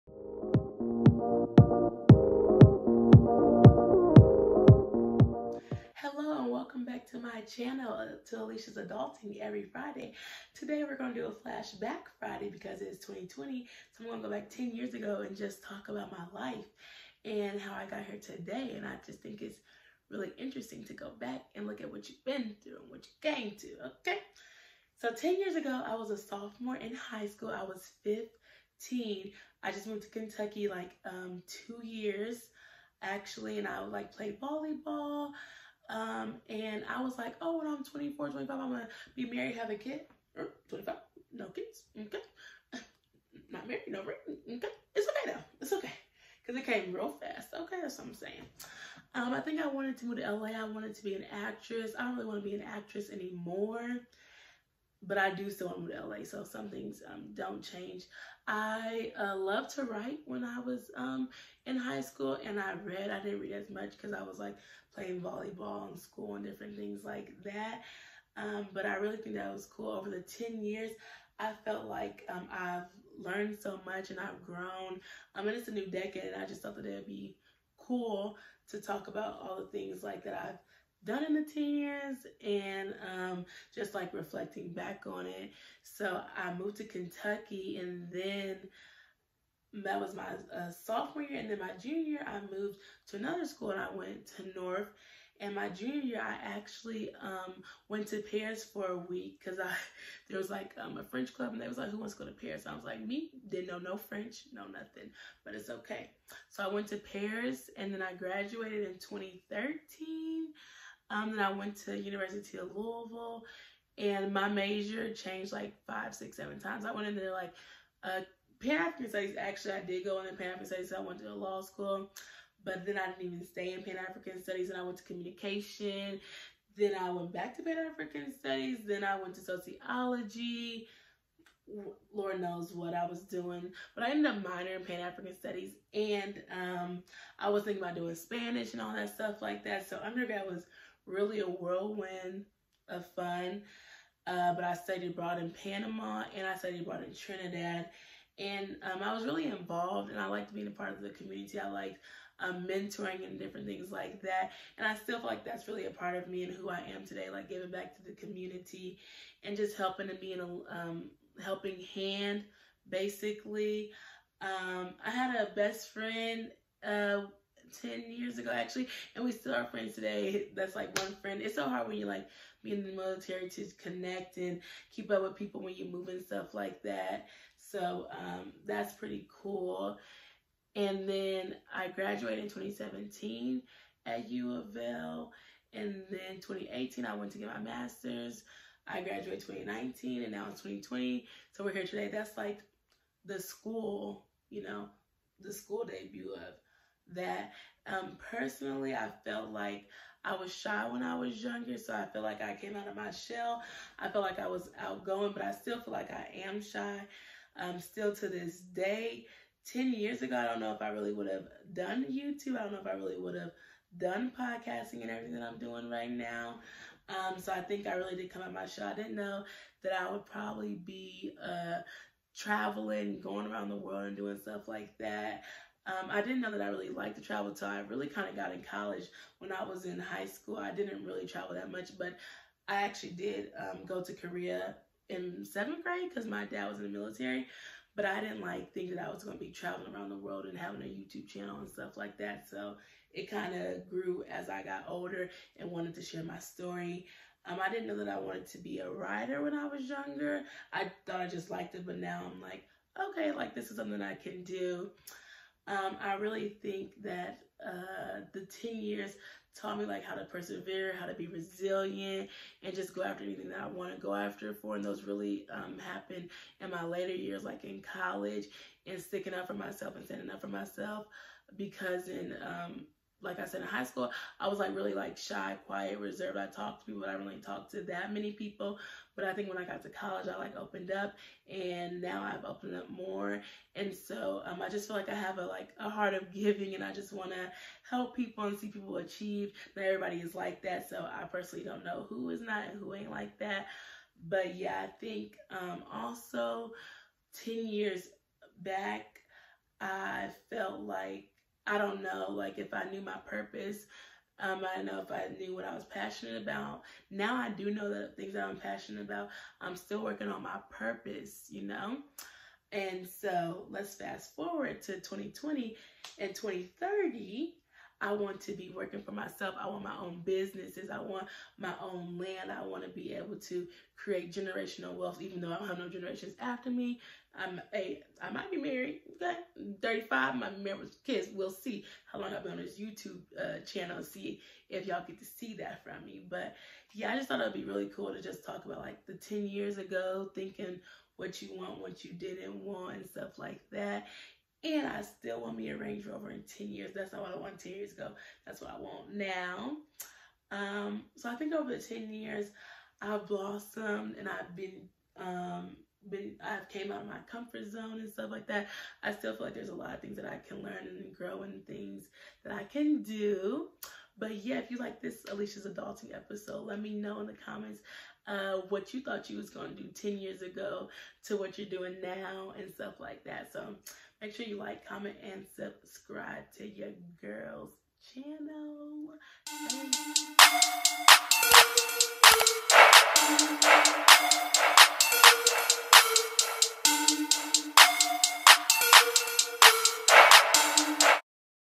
GET TO KNOW ME| NEW YOUTUBER TAG| MY 10 YEAR CHALLENGE |TRAVELING COLLEGE STUDENT|SMALL YOUTUBER TAG
In this video I am doing the #10 year challenge and this is my 100th video whew yess
I’m talking about where I been where I want to go to school.

I’m trying to reach 5000 subscribers and you could help me !!!